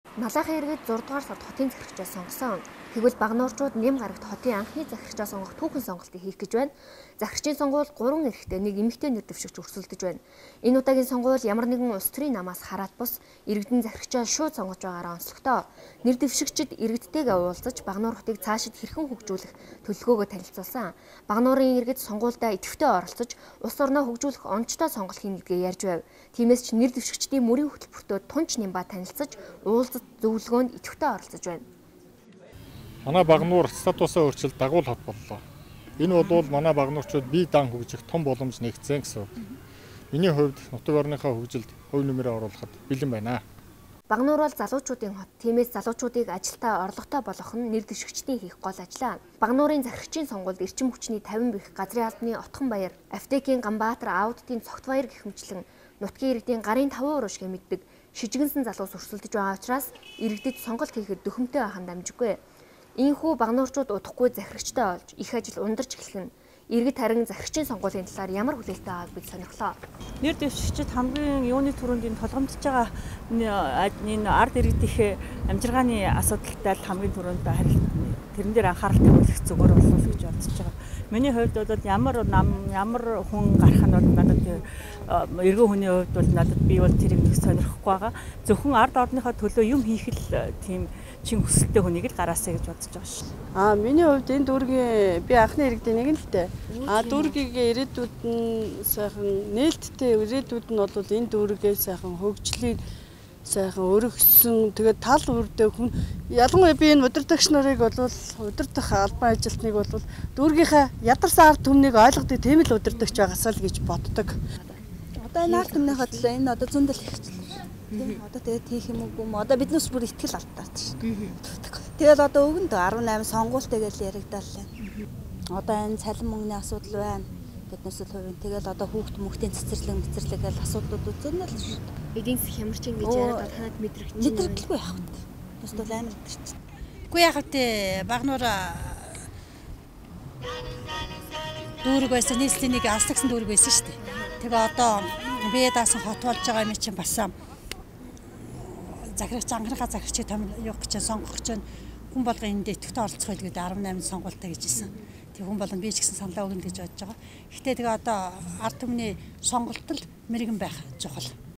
Baganuurchuud anhni zahiragchaa songono
Багануурчууд ням гаригт анхны захирагчаа сонгох түүхэн сонголтыг хийх гэж байна.